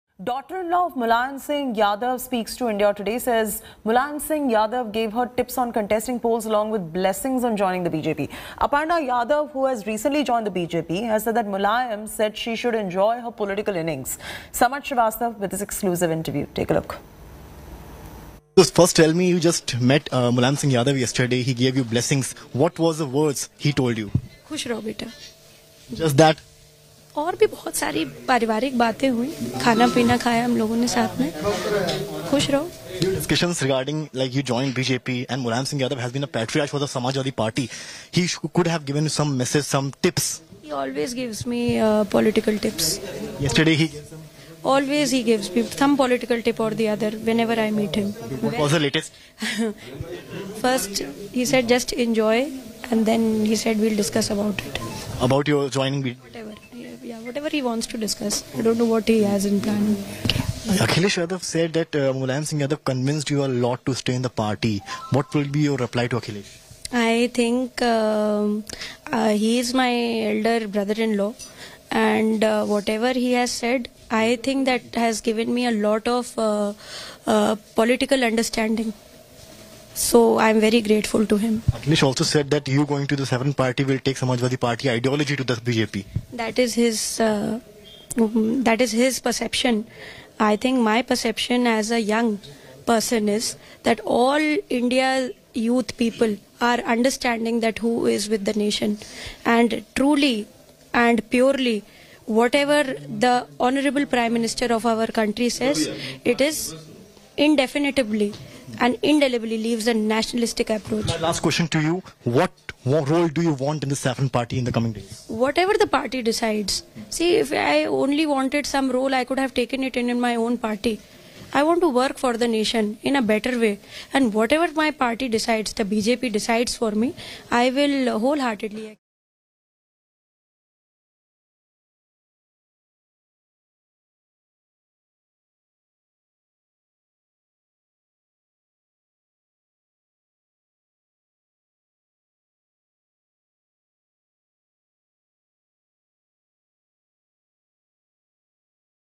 Aparna Yadav: Mulayam Singh Yadav Blessed Me, Gave Tips And Asked Me To Enjoy Politics
Mulayam Singh Yadav's daughter-in-law Aparna Yadav in an exclusive conversation with us said that the veteran politician Mulayam Singh Yadav blessed her, gave tips and told her to enjoy politics.